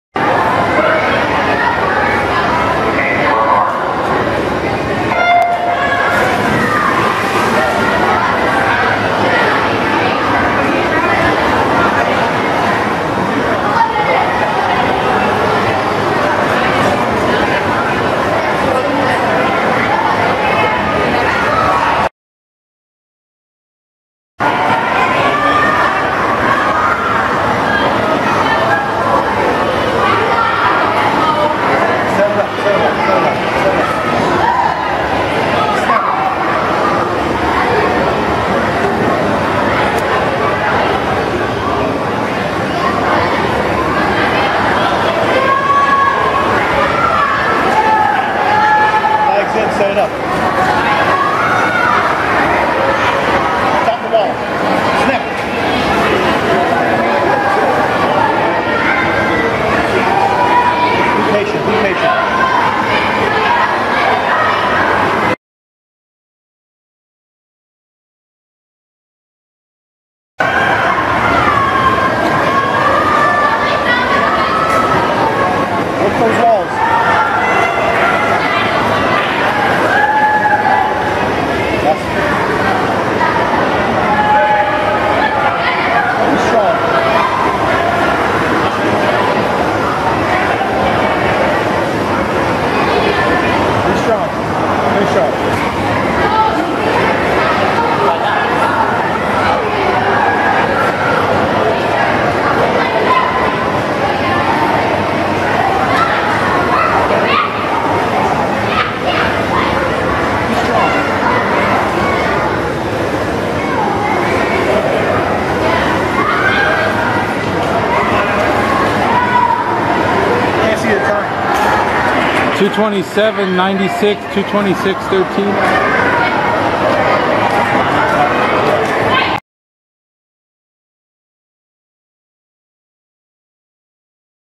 2017 - 07.18 200 Breast, Ale swims in 2:27.94 at Duneland Time Trial
Alejandro swims the 200 Breast on July 18, 2017 in 2:27.94 at the Duneland Time Trial